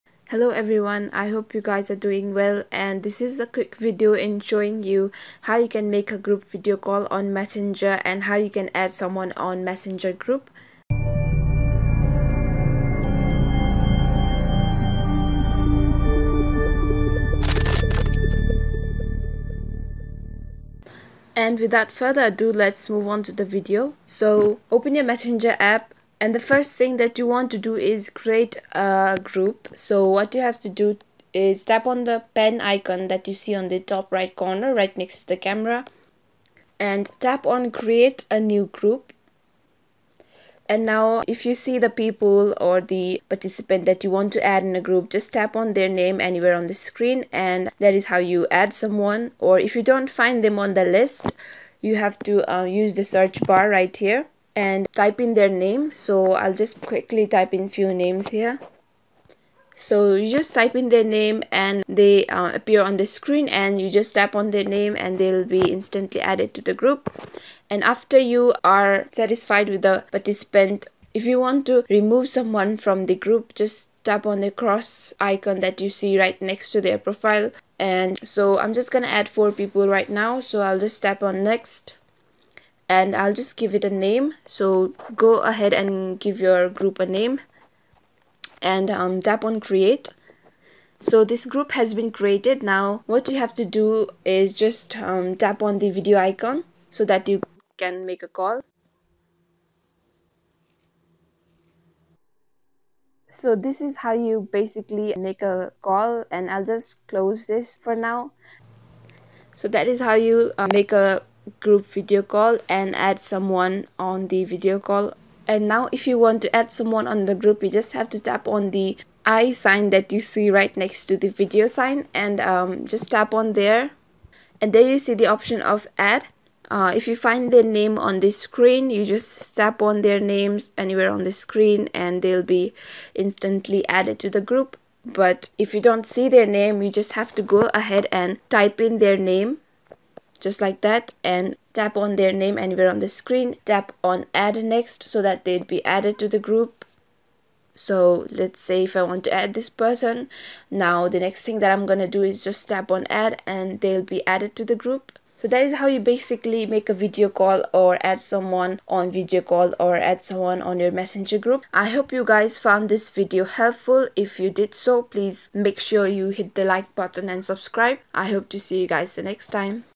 How to Make Group Video Call on Facebook Messenger 2020 | Messenger Conference call
Did you know that you can make a group video call on facebook messenger? This video tutorial walks you through the process involved in how to make group video call on facebook messenger and also how to create a group as well.
This is also known as video conference call where you can call your friends in a single all and talk with all the people on group. However, there are limitation on how many people you can make a call.